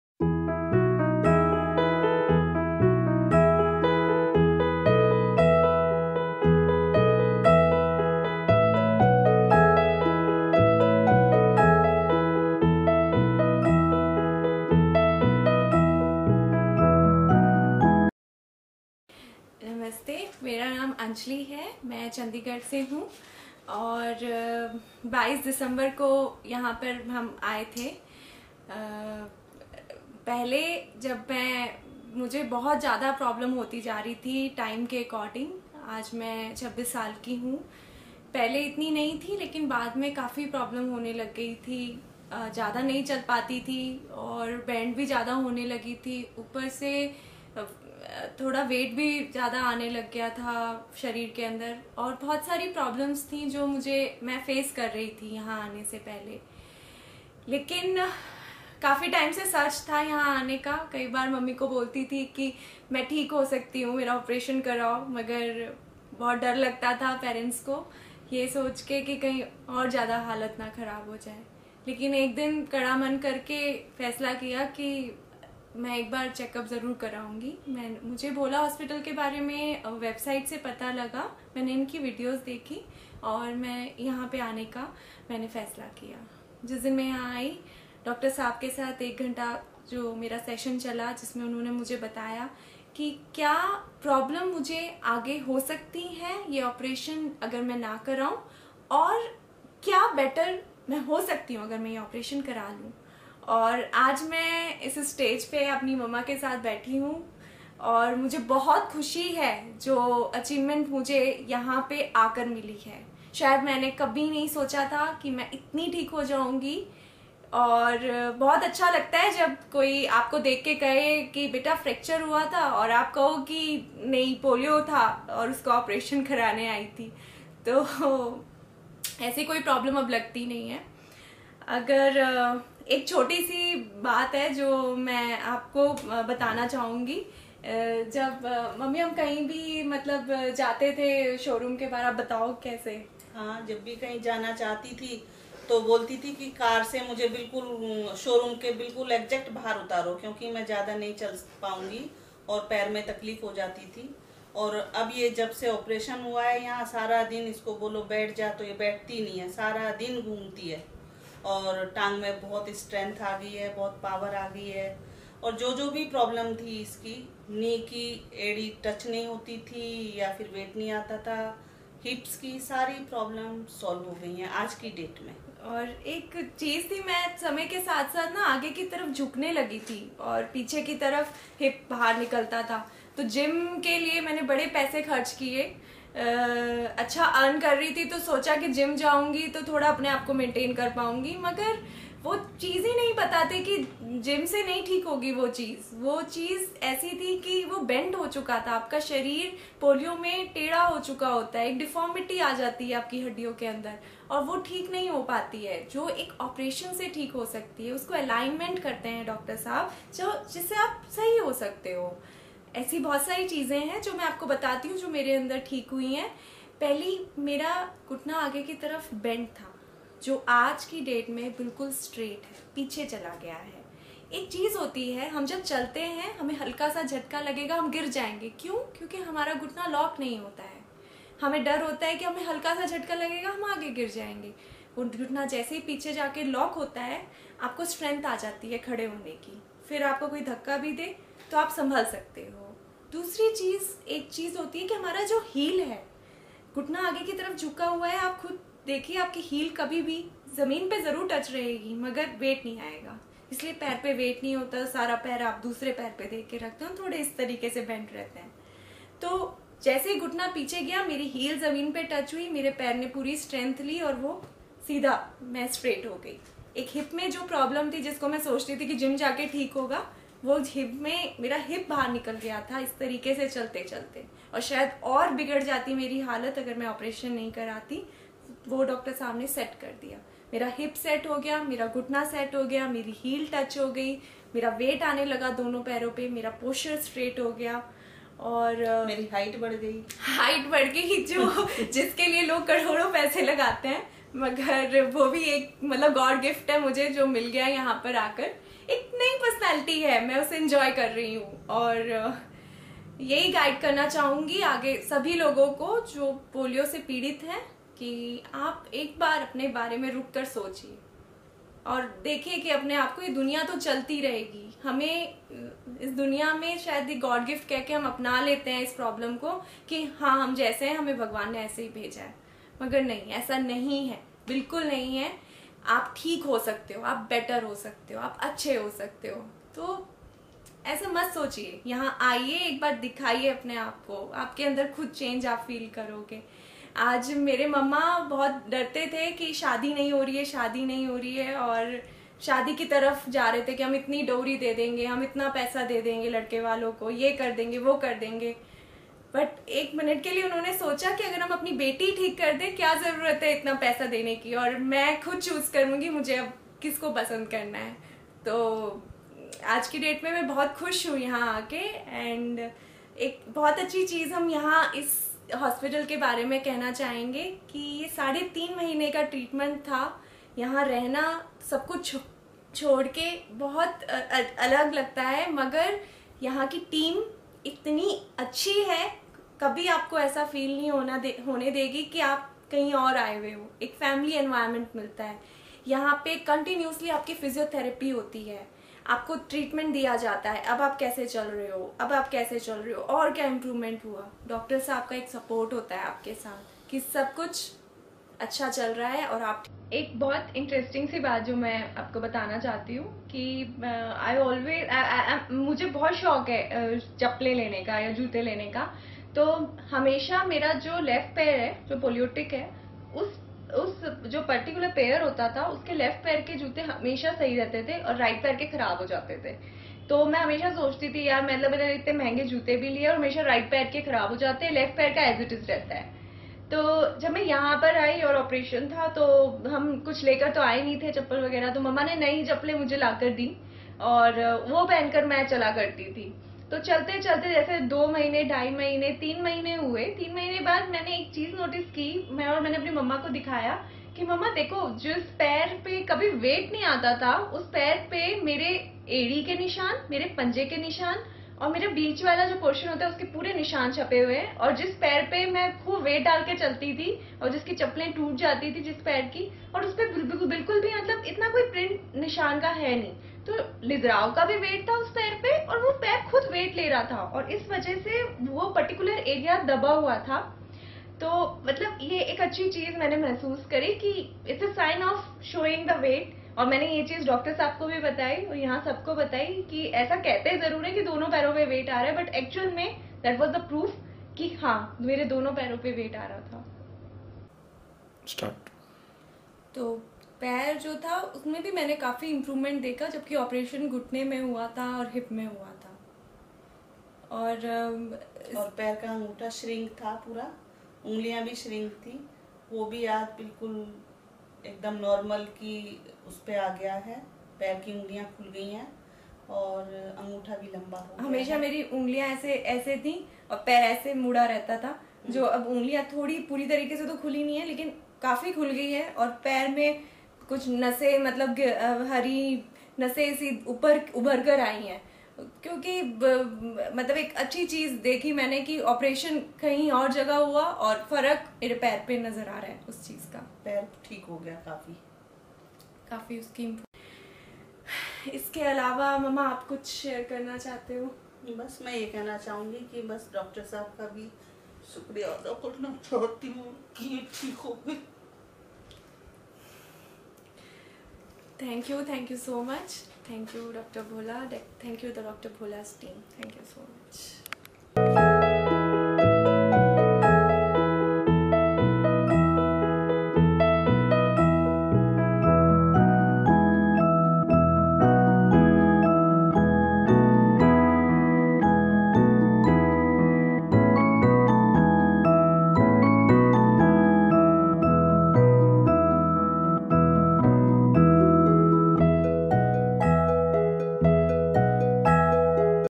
पोलियो क़ी तकलीफों पर इलाज द्वारा जीत
नमस्ते। मैं अंजलि ,चंडीगढ़ क़ी रहने वाली हूँ। 22  दिसंबर 2015 को मैं भोला अस्पताल के लिए आई थी  मैं 26 साल क़ी हूँ। मेरी समस्या उम्र के साथ बढ़ती जा रही है । मेरा वजन बढ़ने क़ी वजह से आगे कीतरफ झुकाव भी बढ़ रहा था मैं लंबी दूर से नहीं चल पाती थी । मुझे इस जगह पर आने से पहले कई समस्याओं का सामना करना पड़ा । मैंने अपने इलाज के लिए मेरी माँ को बताया। लेकिन माता-पिता भयभीत है कि चीजें खराब नहीं हो सकती थी। लेकिन एक दिन मैं यूट्यूब और वेबसाइट पर खोज करने के बाद डॉ भोला को दिखाने के लिए अपना मन बना लिया। पहले दिन, मैं  एक घंटे के लिए डॉ भोला के साथ बैठक की थी। उन्होंने कहा कि मुझे भविष्य में और गंभीर समस्याओ का सामना करना पड़ सकता है  । आज मैं अपनी मां के साथ बैठी  हूँ । मैंने कभी नहीं सोचा था कि मुझे आपरेशन  होने के बाद इतना लाभ प्राप्त होगा। अब वास्तव में किसी को पता नी लगता क़ी पोलियो से पीड़ित थी हूँ ,अच्छा लगता है। इससे पहले, जब भी हम बाजार के लिए जाते थे  , पहले हम जब भी शॉपिंग करने के बाहर जाते थे तो बहुत दूर चलना पड़ता था पार्किंग में कार लगाने के बाद , । मैंने पहले जिम पे भी बहुत खर्च किया परन्तु असली समस्या डॉ सुरेश भोला से बात करने के बाद पता चली ।  मुझे सर्जरी करने के बाद बहुत लाभ मिला ।
मेरी माँ मेरे शादी के बारे में, बहुत चिंतित थी । लेकिन वह मेरे इलाज के बारे में नहीं सोचा था। आज मैं अपने जीवन साथी अपने आप चुन सकती हूँ । मैं अपने बारे में बहुत खुश हूँ। मेरे इलाज के तीन और एक आधे महीने के लिए चला । यह बहुत मुश्किल है इस तरह के एक लंबे समय के लिए रहने के लिए घर से दूर है, लेकिन यहाँ पर वातावरण बहुत घरेलू था। मैं जूते की नई जोड़ी क़ी बहुत शौकीन हूँ। मेरे बाएं पैर में समस्या थी इसलिए जूता हमेशा बहुत जल्द ही क्षतिग्रस्त हो जाया करता था।
जब मैं अस्पताल के लिए आई मैंने एक नई जोड़ी खरीदी थी ।  एक ही जोड़ी चलने के लिए इस्तेमाल किया। आश्चर्य क़ी  बात, 3 महीने के बाद मैंने पाया कि मेरे दोनों जूते सही थे । मेरी पैर क़ी उंगलिया जो पहले  मुड़ी हुई थी वो भी सीधी हो ।
अंत में मैं डॉ भोला और उनकी टीम धन्यवाद करती हूँ।

यदि आप किसी भी प्रश्न है, तो आप 6.00 बजे शाम को  पर मुझसे बात कर सकते हैं।  अस्पताल नंबर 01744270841 और मोबाइल नंबर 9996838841, रविवार को छोड़कर?